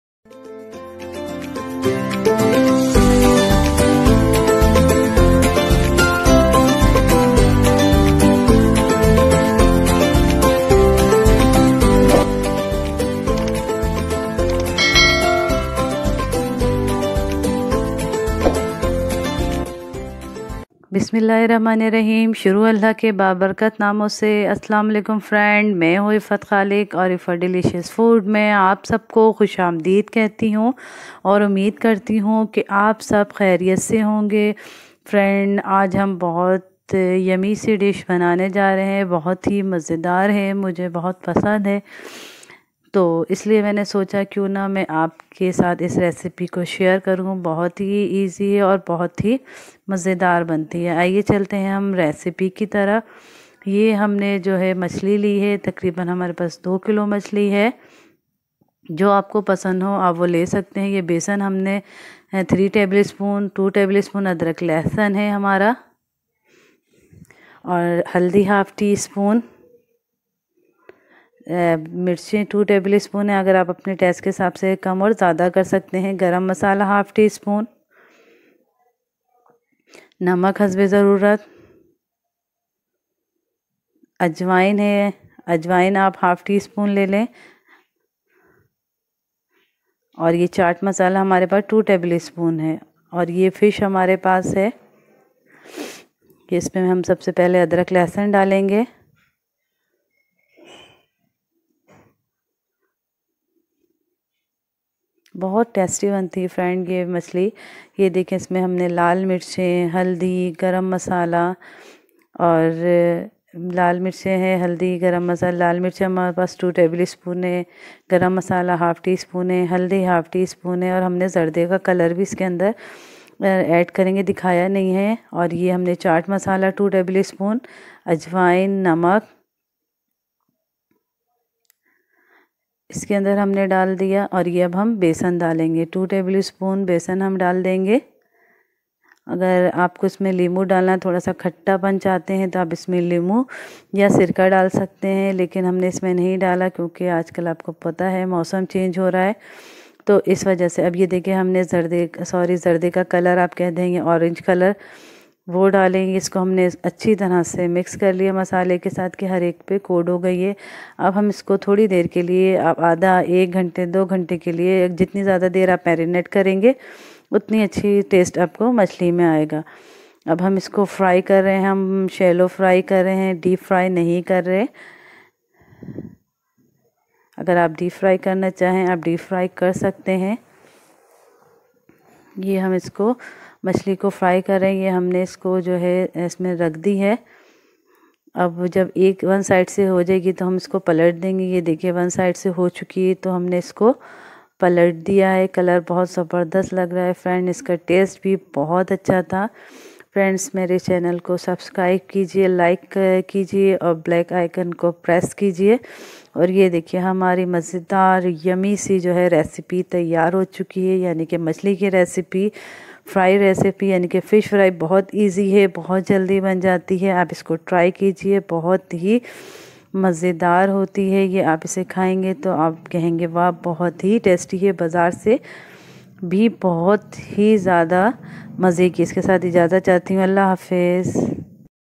**Fish Fry**||Fish Garvy|| Mere Style Main
Assalam o Alikum friends kesay hai ap sb log aj ham massage dar yummy qeeme ki recipe laye kar aye hai jo bohat hi mazedar banta hai